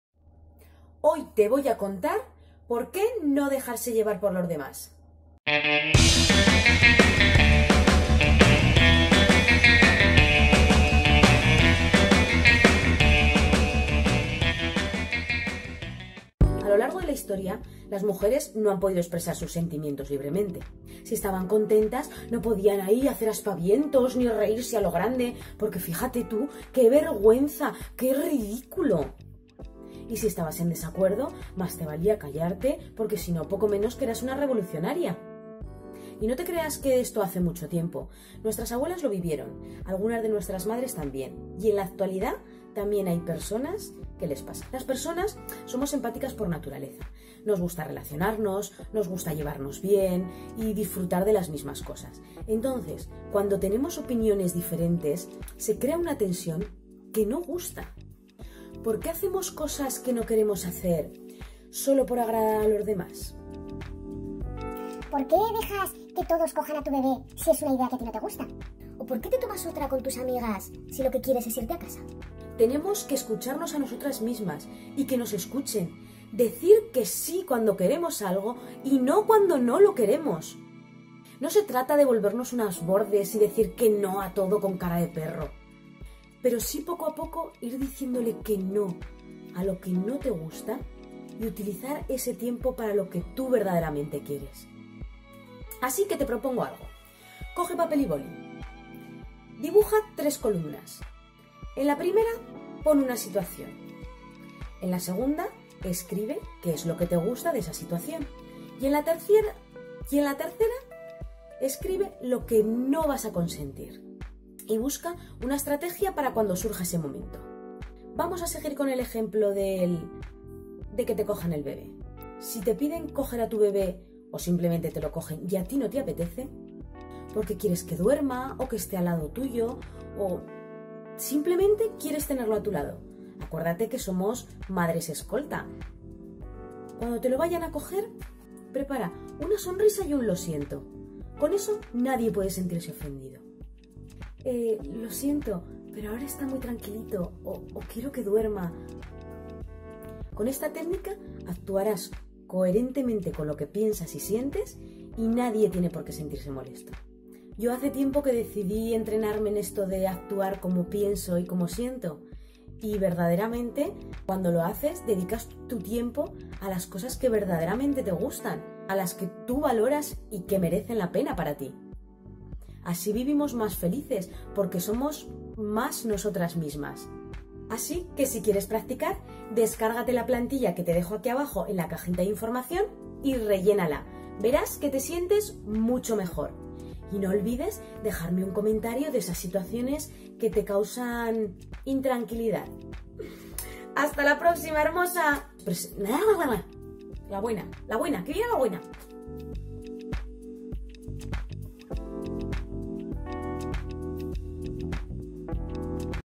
SER COHERENTE CON TUS IDEAS / DECIR NO
A veces es dificil ser coherente con tus propias ideas, sobre todo cuando el entorno no opina como tú.
Pero puedes encontrar la felicidad, aprendiendo a decir no a lo que no te gusta.
Cada vez que actuas como piensas y sientes, comienzas a ser mas tu misma.
El ser humano es empático por naturaleza. Le gusta relacionarse e interacturar con el resto, y en un esfuerzo por conseguir ésto, podemos acceder a hacer cosas o consentir otras que no nos gustan demasiado.
Cuantas veces hemos hecho algo porque nos lo han pedido y luego nos hemos sentido incómodas por ello?
¿Porqué seguimos teniendo comportamientos "socialmente correctos" cuando no estamos de acuerdo?
Y es que es muy importante hacernos oir. Y mas importante, oirnos a nosotras mismas.
Atrevernos a decir que SI cuando estamos de acuerdo en algo y NO cuando no lo estamos.
No dejar que nuestra respuesta o actitud se rija por el comportamiento o la opinión de los demás o por complacer a alguien.
Cuando lo haces dedicas tu tiempo a cosas que valoras y que a tí te merecen más la pena. Tu tiempo es limitado, APROVÉCHALO en lo que tu quieres y con los que tu quieres.
Aprendiendo ésto, vivimos más felices, porque somos nosotras mismas, nos sentimos mejor, y cuando ésto ocurre, estamos más contentas, más fuertes y todo sale mejor, y eso nos genera más felicidad.